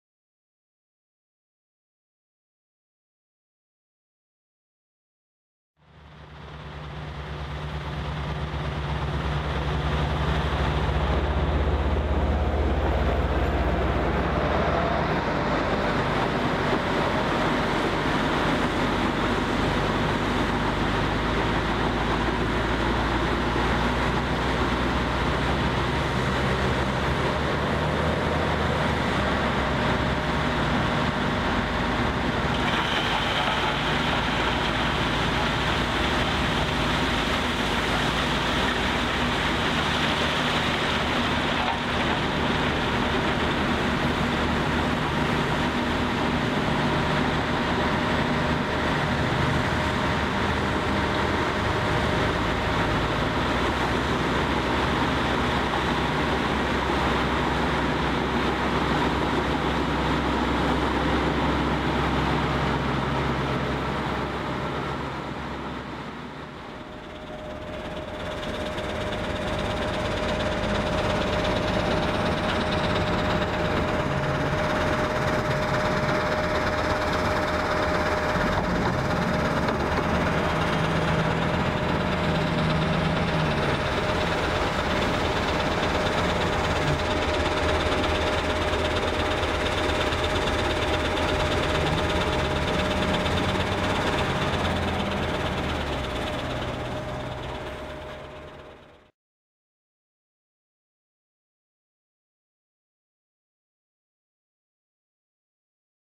Volvo L220E & Volvo L220G Working
Volvo L220E From Poms Moving Gravel In A Quarry.
Volvo L220E Från Poms Flyttar Grus I En Bergtäkt.

Volvo L220G From RCG Moving Gravel  In A Quarry.
Volvo L220G Från RCG Flyttar Grus I En Bergtäkt.